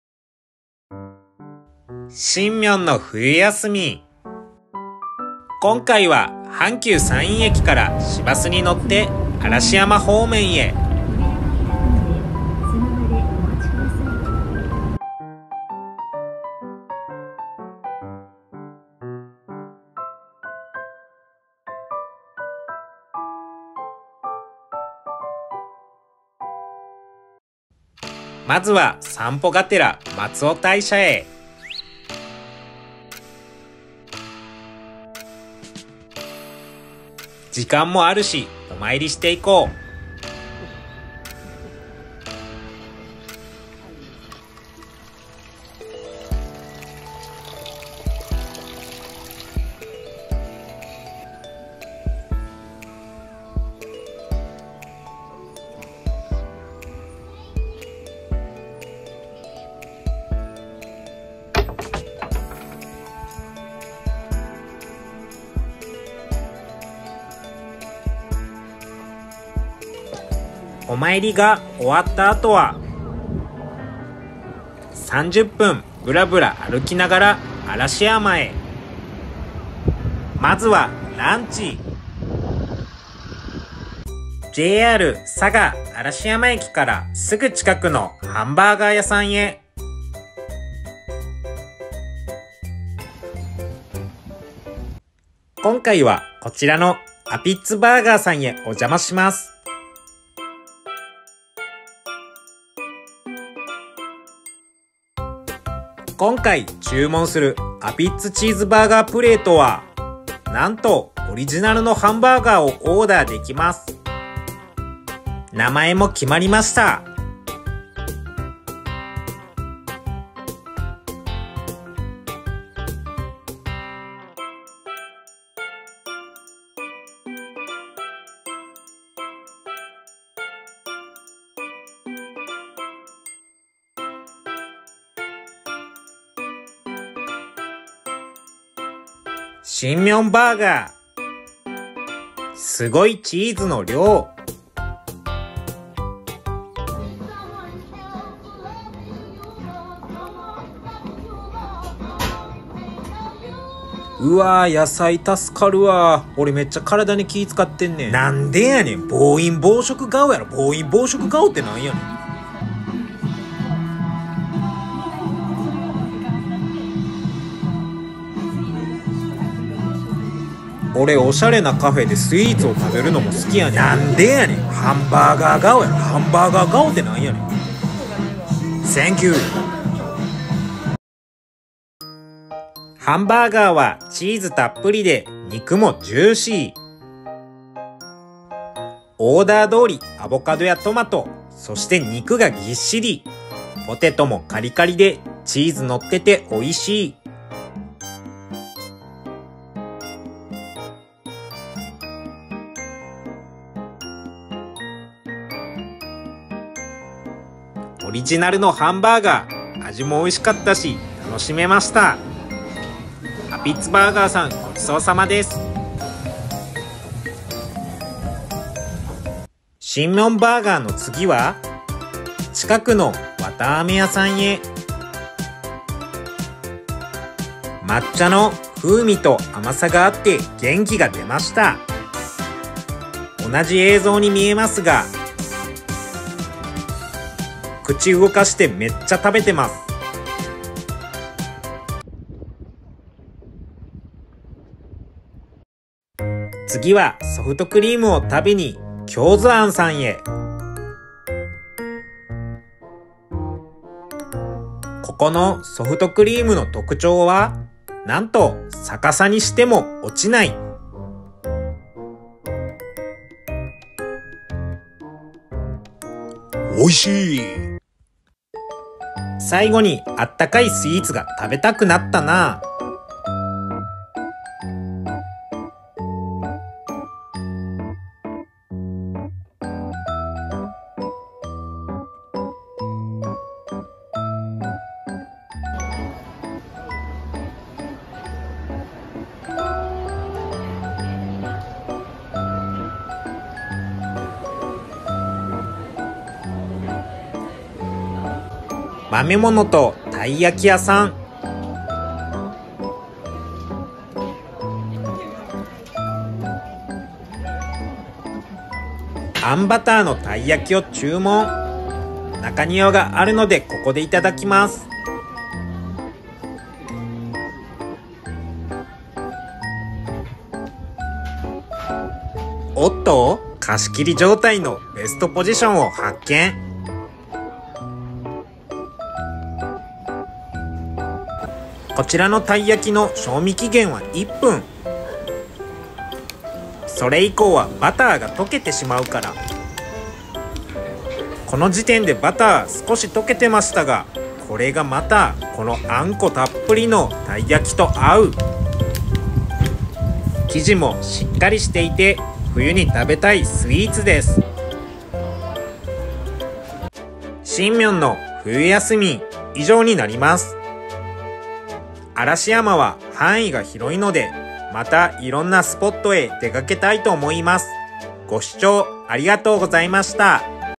しんみょんの冬休み2023 嵐山グルメ
【upit's Burger】

〒616-8373 京都府京都市右京区嵯峨天龍寺車道町４−７
11:00〜20:00

【zarame -gourmet cotton candy-】

〒616-8373 京都府京都市右京区嵯峨天龍寺車道町1
10:00〜18:00 土日9:00〜19:00

【手作り工房 京豆庵 嵯峨嵐山店】

〒616-8375 京都府京都市右京区嵯峨天龍寺立石町２−１
10:00〜17:00

【まめものとたい焼き】

〒616-8385 京都府京都市右京区嵯峨天龍寺芒ノ馬場町４０−８ 昇龍苑 １Ｆ
10:00〜17:00